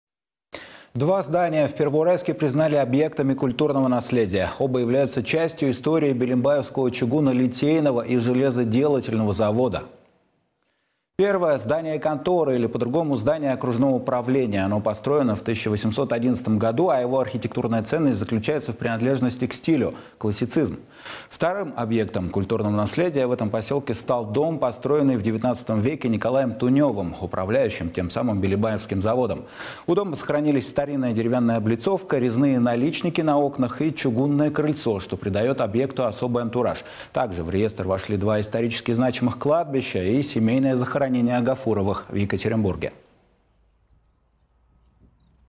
Два здания в Первоуральске признали объектами культурного наследия
Два здания в Первоуральске признали объектами культурного наследия. Оба являются частью истории Билимбаевского чугунолитейного и железоделательного завода.
Первое - здание конторы, или по-другому «Здание окружного правления». Оно построенно в 1811 году, а его архитектурная ценность заключается в принадлежности к стилю – классицизм. Вторым объектом культурного наследия в этом посёлке стал дом, построенный в 19 веке Николаем Тунёвым – управляющим тем самым Билимбаевским заводом. У дома сохранились старинная деревянная облицовка, резные наличники на окнах и чугунное крыльцо, что придаёт объекту особый антураж. Также в реестр вошли два исторически значимых кладбища и «Семейное захоронение Агафуровых» в Екатеринбурге.